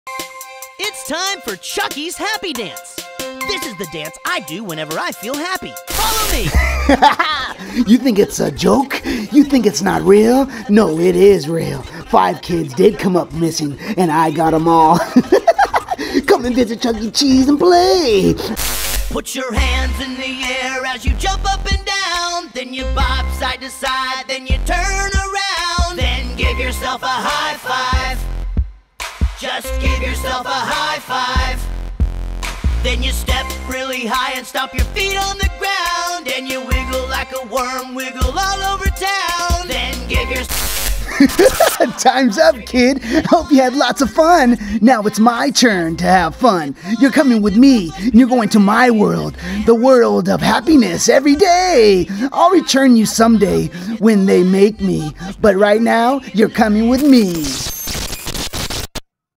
DID 5 KIDS REALLY GO MISSING FROM CHUCK-E-CHEESE? I FOUND SOME OLD FOOTAGE THAT MAY HAVE THE ANSWERS
Are the rumors true? Did kids go missing from Chuck-E-Cheese. I found this creepy video of 1 kids who went missing. Luckily he was found, and he is safe at home now. You decide for yourself, Rumor or Fact? I want to believe it isn't true. Did 5 kids really go missing from Chuck E Cheese?